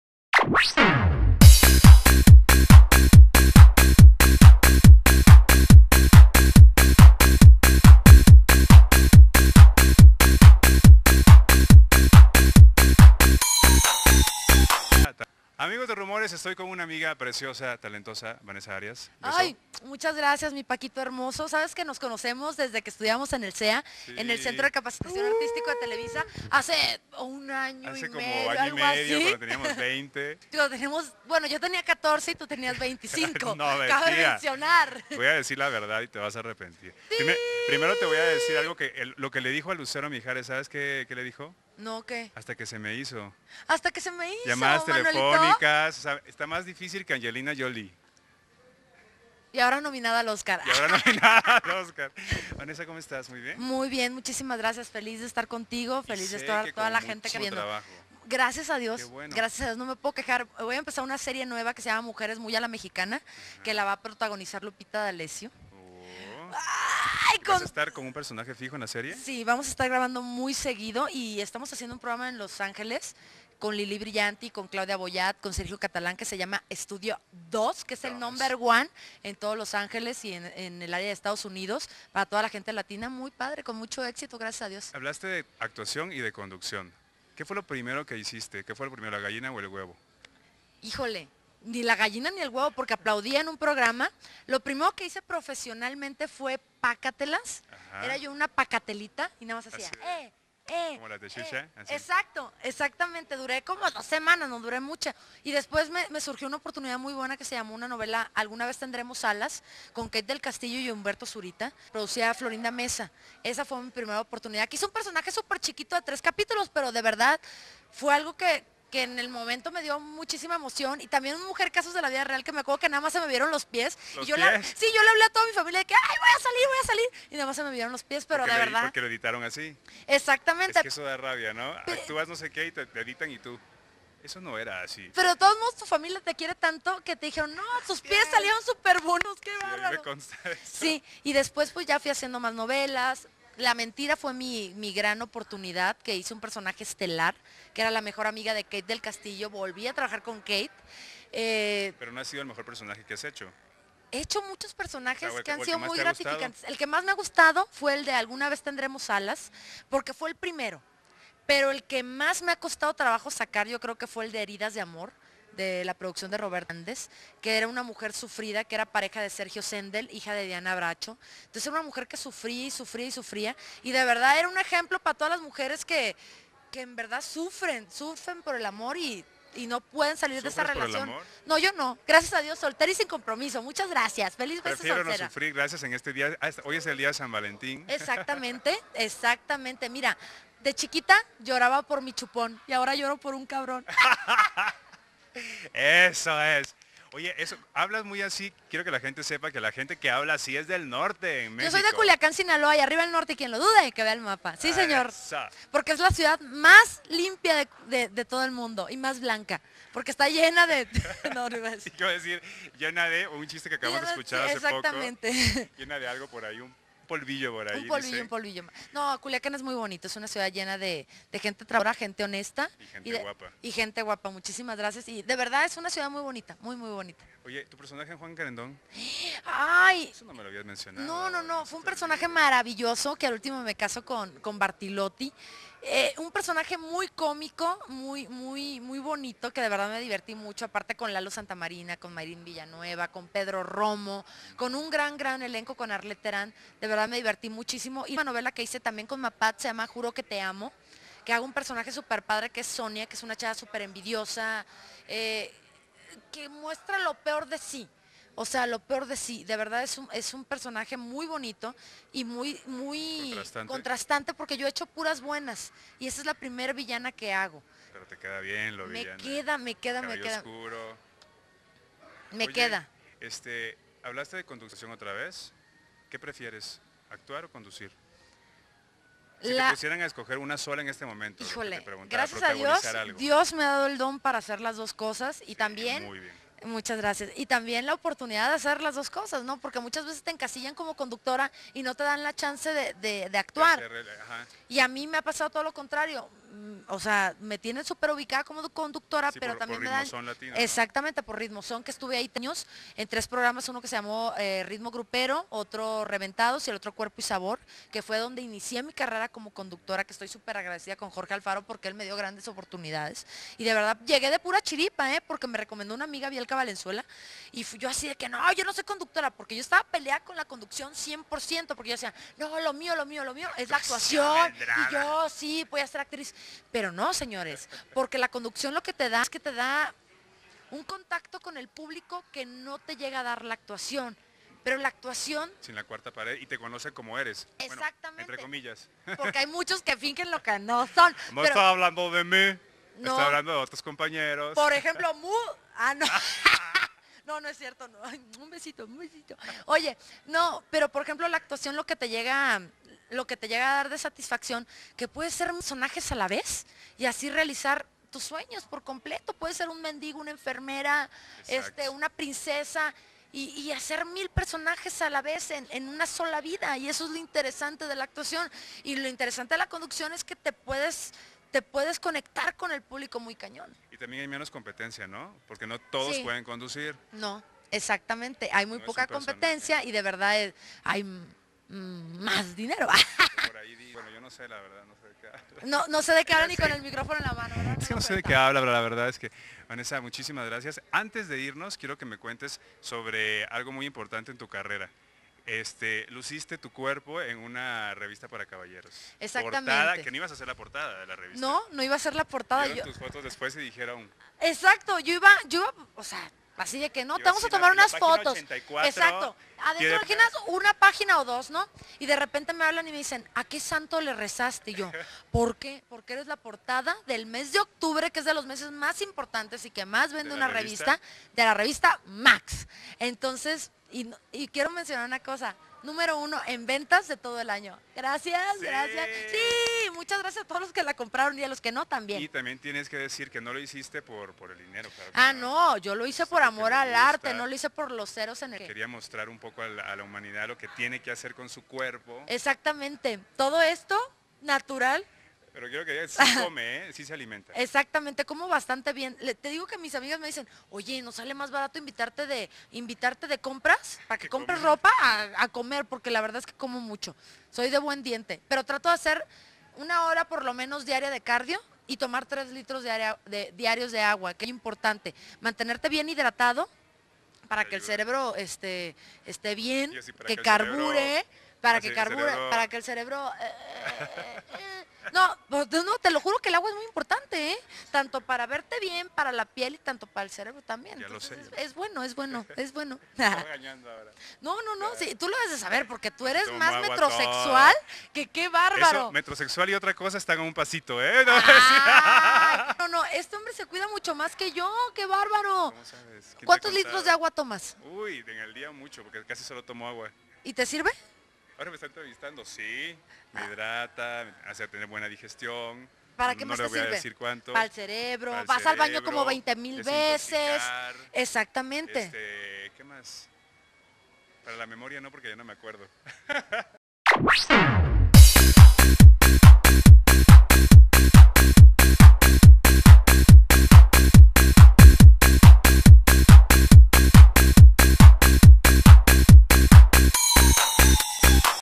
VANESA ARIAS (ENTREVISTA CON FRANCISCO CALVILLO)
VANESA ARIAS (ACTRIZ Y CONDUCTORA-PORTADA REVISTA MAX-OCTUBRE 2008)  CONDUCTORA DE "ESTUDIO 2" (LOS ANGELES). ENTREVISTA CON FRANCISCO CALVILLO. MÉXICO D.F.